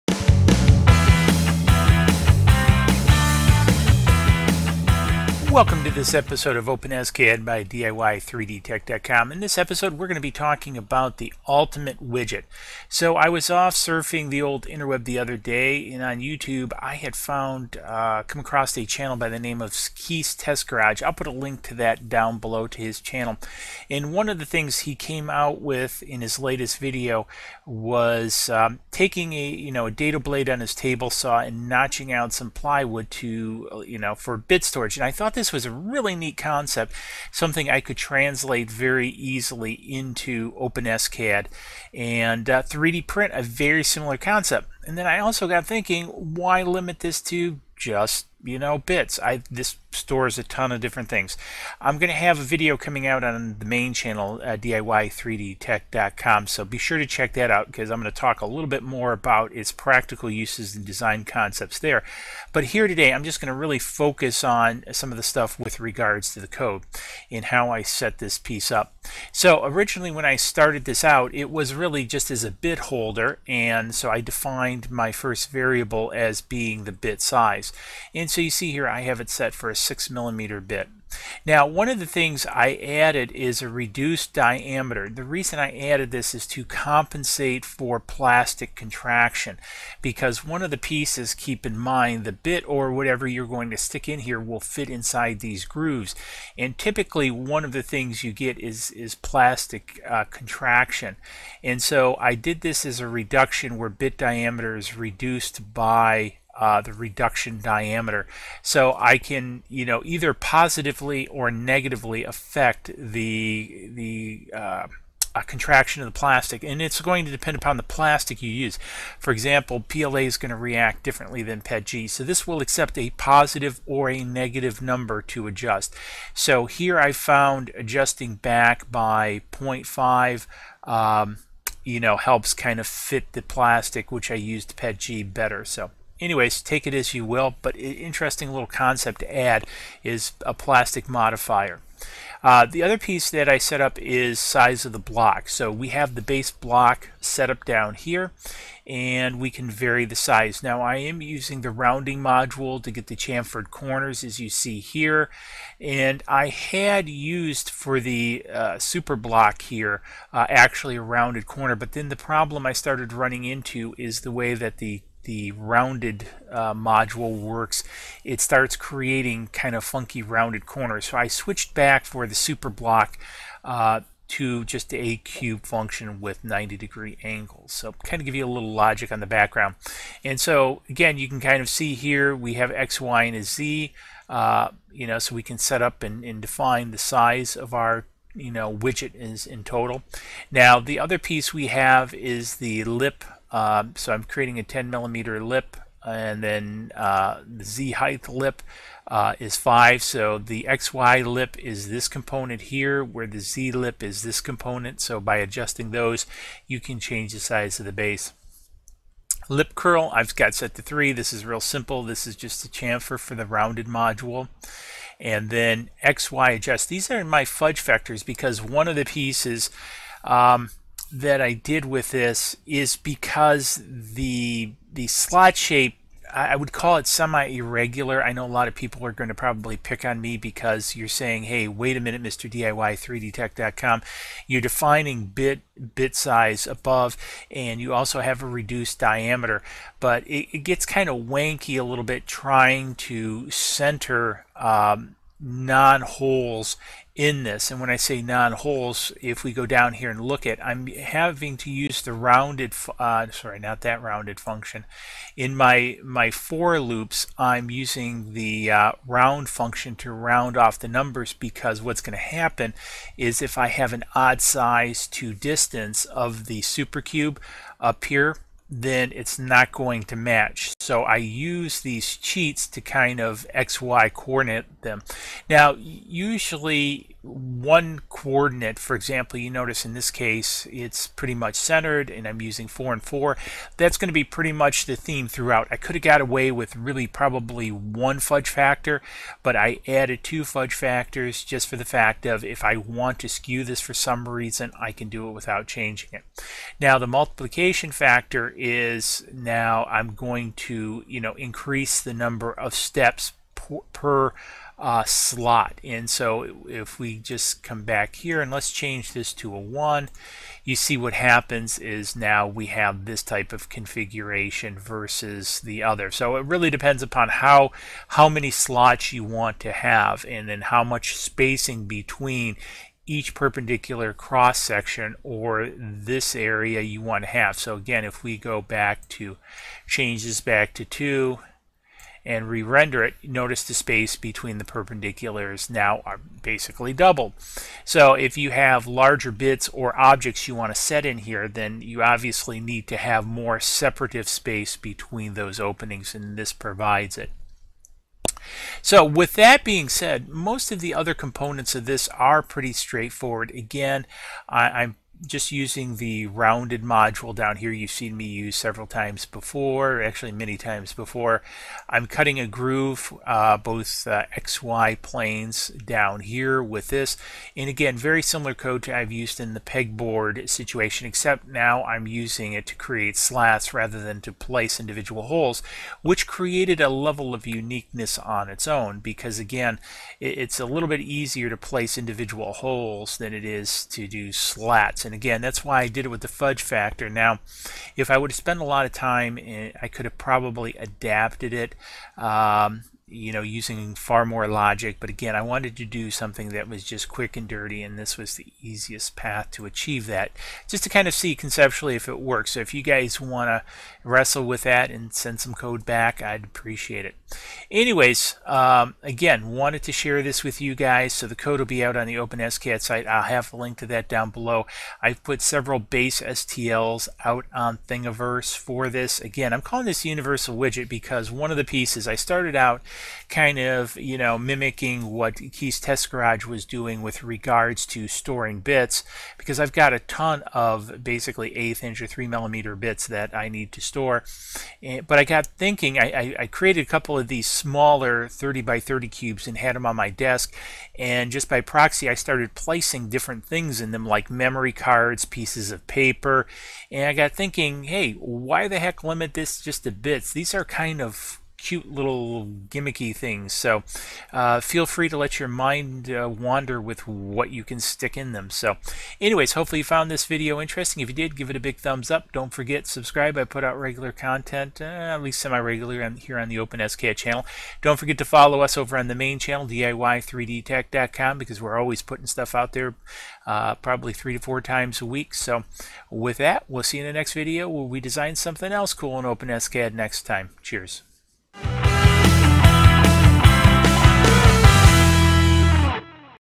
Open SCAD - 3D Printing the Ultimate Widget!
for more information on this and many other projects! As in this episode we will be looking at programing the Ultimate Widget as I was surfing the YouTube InterWeb and found Keith's Test Garage ) and a video he created caught me eye (this one ) and I got thinking, I can design this in OPEN-SCAD and also why does it need to be limited to only router bits!  So lacking a name, the ultimate widget was born!

Looking for the best deals on 3D Printers or maybe just want to see what new, this link is for you

For more information as well as the complete source code, please

I have put several STL of this design on Thingiverse